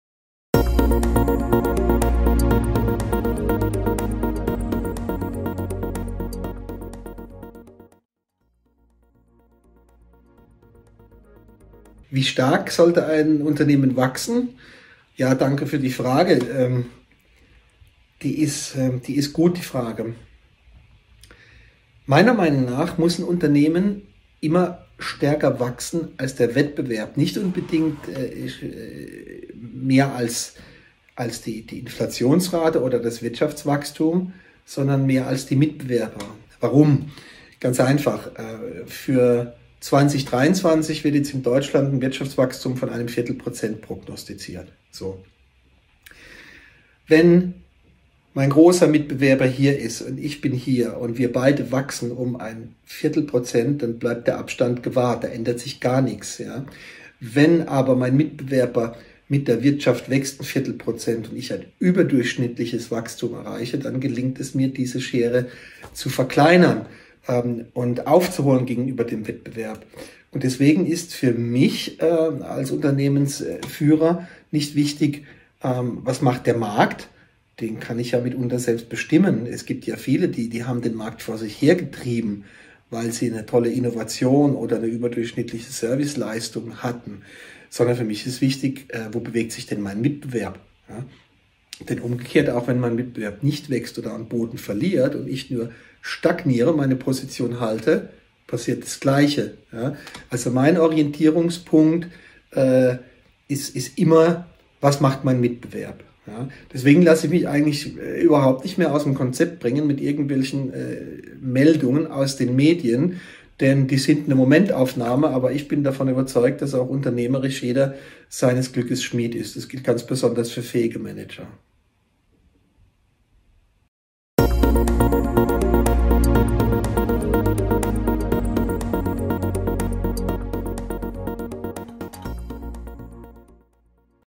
Wieviel Wachstum braucht ein Unternehmen? Inflation, Wirtschaftswachstum, welcher KPI ist richtig?
Unternehmen müssel wachsen und sie müssen Wachstum planen. Aber wieviel ist richtig und auch umsetzbar? An was sollen sich Geschäftsführer und Vorstände orientieren? Welche Geschäftsstrategie, welches Unternehmenswachstum ist richtig und umsetzbar? An was soll man sich orientieren?

Uwe Rembor, einer der erfolgreichsten Restrukturierern Deutschlands und mehrfach ausgezeichneter Interim Manager, hat da eine klare Empfehlung.